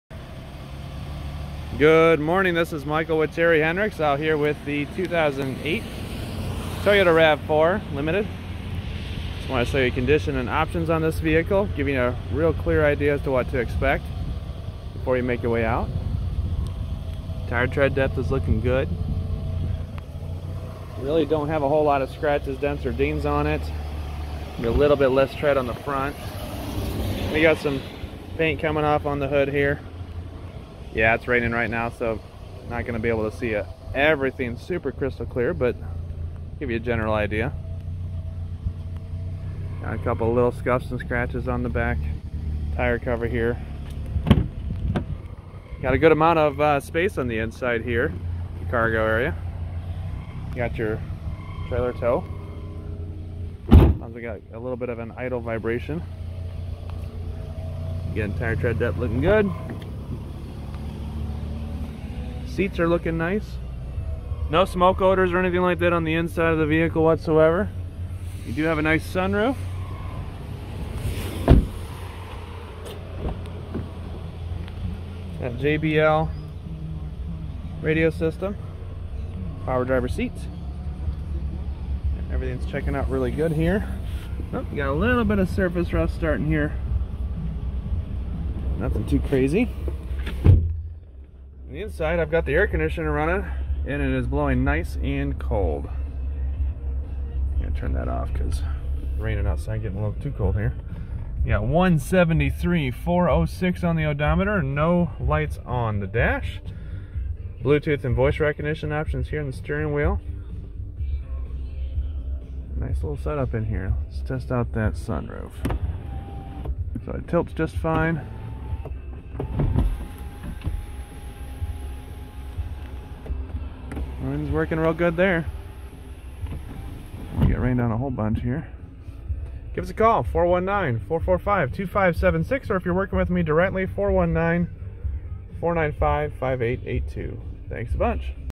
Used 2008 Toyota Rav4 Limited Walkaround Video
2008 Rav4 Walkaround Video Terry Henricks